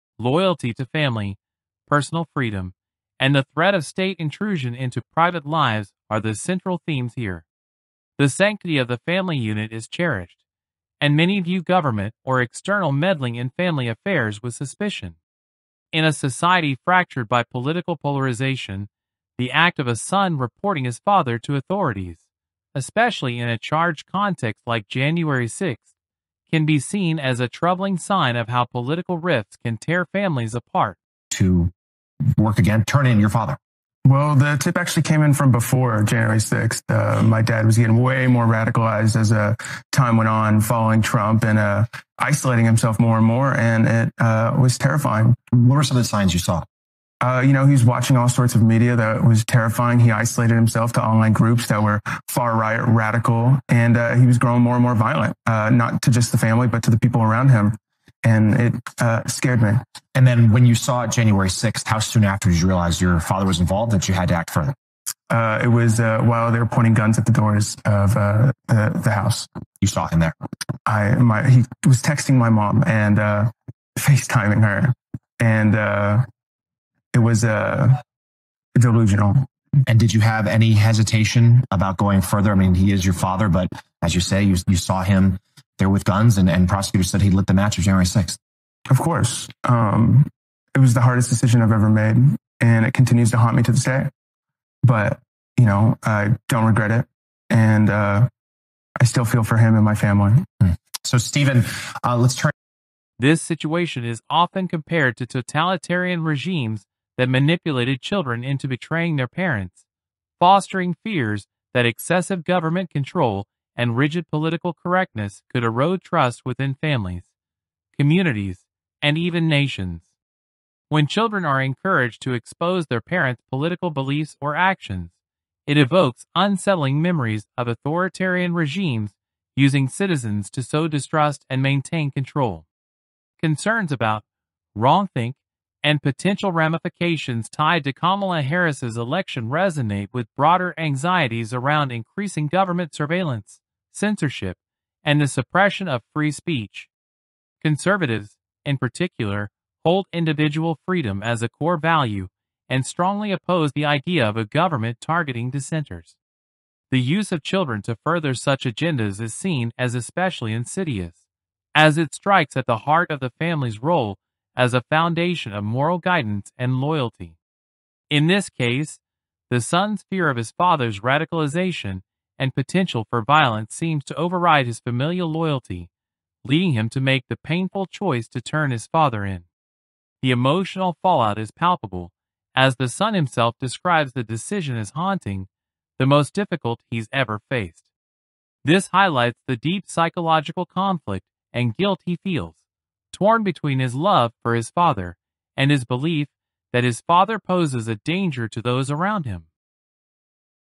Son Explains Decision to Turn in His Father Over January 6th Involvement
In this powerful MSNBC interview, a son shares the heart-wrenching decision he made to report his father for involvement in the January 6 Capitol riots. The son explains how his father became increasingly radicalized, isolated, and violent in the lead-up to the insurrection. Despite the emotional weight of turning in his own parent, he stands by his decision and discusses why he doesn’t regret it. This interview offers a gripping look into the fallout of Jan 6, the impact of political extremism on families, and how some are confronting radicalization even within their own homes.